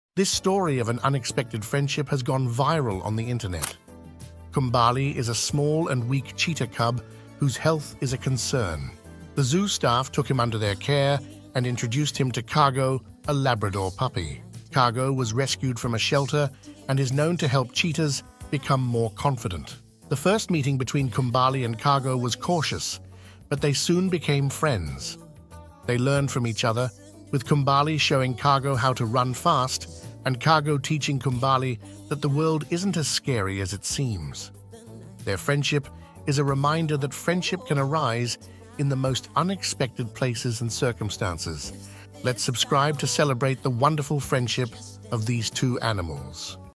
Cheetah And Lab Are Besties!
Get ready to witness the most adorable friendship that's taking the internet by storm! 🌍💫 Meet Kumbali, the fragile cheetah cub fighting for his health, and Cargo, the courageous Labrador puppy with a heart as big as his bark. 🐾🐕‍🦺

When Kumbali needed a friend, Cargo stepped up, showing that true friendship knows no species. Watch as they embark on a journey of learning, playing, and growing together. From their first cautious encounter to becoming inseparable buddies, this duo proves that sometimes, the friend you need comes in the most unexpected form. 🐆🐶

Their story is more than just cute; it's a powerful reminder of resilience, hope, and the magic of companionship. Kumbali learns the world isn't so scary with a friend by his side, while Cargo discovers the thrill of running like the wind. 🌬️❤️

Let's spread the message that friendship can bloom anywhere and change lives in the most beautiful ways.